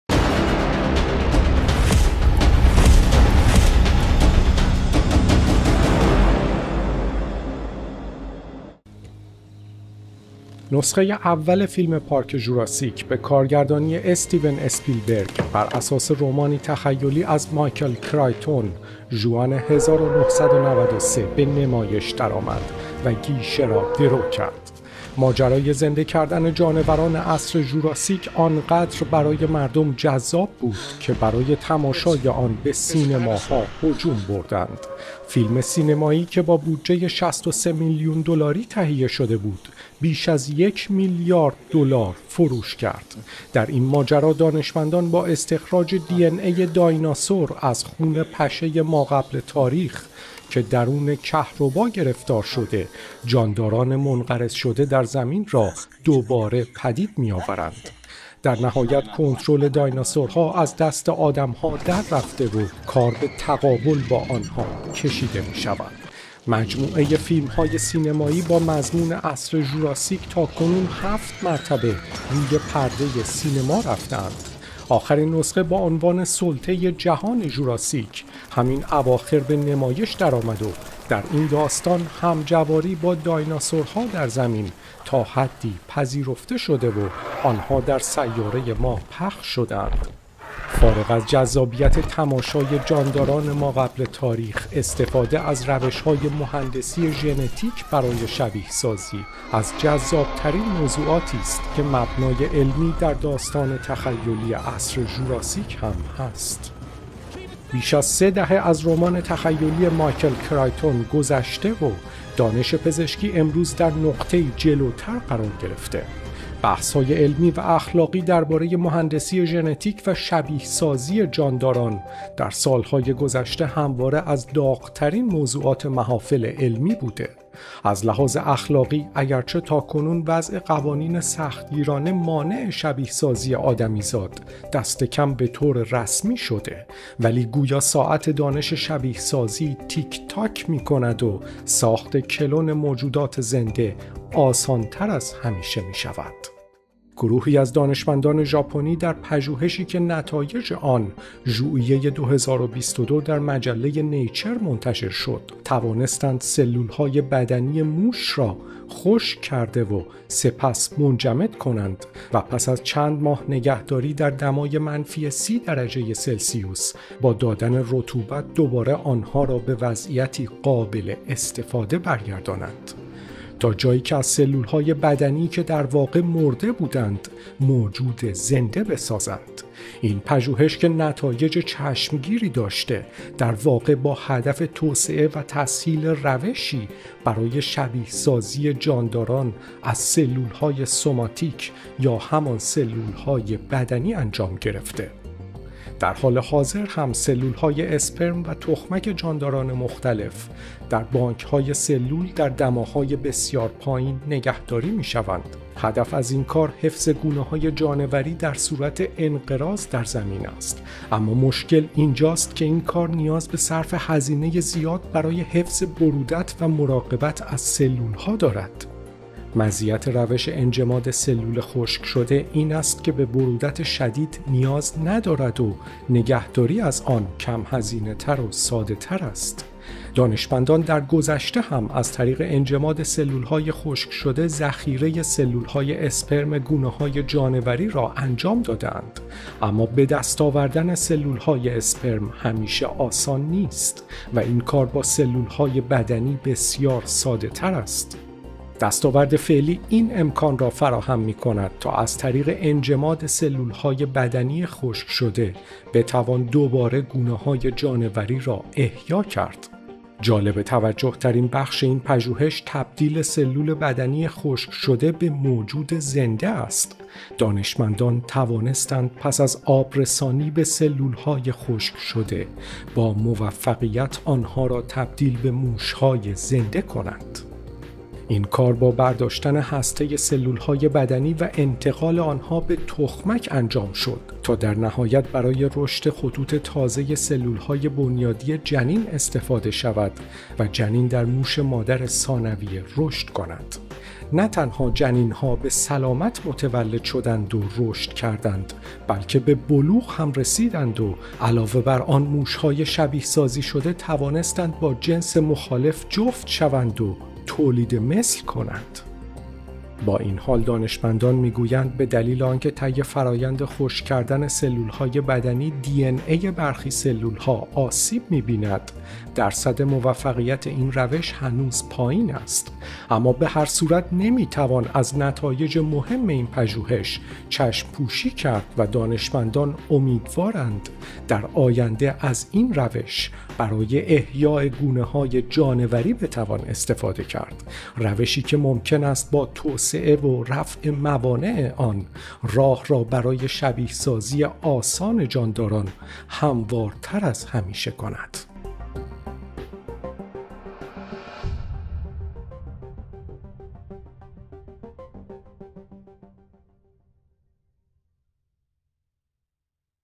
کیهان لندن | زنگ خطر؟ ژاپنی‌ها از سلول خشک‌شده موجود زنده ساختند
فرید خلیفی – نسخه اول فیلم «پارک ژوراسیک» به کارگردانی استیون اسپیلبرگ بر اساس یک رمان تخیلی از مایکل کرایتون در ژوئن ۱۹۹۳ به نمایش درآمد و گیشه را درو کرد! ماجرای زنده کردن جانوران عصر ژوراسیک آنقدر برای مردم جذاب بود که برای تماشای آن به سینماها هجوم بردند. فیلم سینمایی که با بودجه ۶۳ میلیون دلاری تهیه شده بود، بیش از یک میلیارد دلار فروش کرد. در این ماجرا دانشمندان با استخراج دی‌ان‌ای دایناسور از خون پشه ماقبل تاریخ که درون کهربا گرفتار شده بود، جانداران منقرض شده در زمین را دوباره پدید می‌آورند. در نهایت کنترل دایناسورها از دست آدم‌ها در رفته و کار به تقابل با آنها کشیده می‌شود. مجموعه فیلم‌های سینمایی با مضمون عصر ژوراسیک تا کنون ۷ مرتبه روی پرده سینما رفته‌اند. آخرین نسخه با عنوان «سلطه جهان ژوراسیک» همین اواخر به نمایش درآمد و در این داستان همجواری با دایناسورها در زمین تا حدی پذیرفته شده و آنها در سیاره ما پخش شده‌اند. فارغ از جذابیت تماشای جانداران ماقبل تاریخ، استفاده از روش‌های مهندسی ژنتیک برای شبیه‌سازی موجودات زنده از جذاب‌ترین موضوعاتی‌ست که مبنای علمی در داستان‌های تخیلی عصر ژوراسیک هم هست.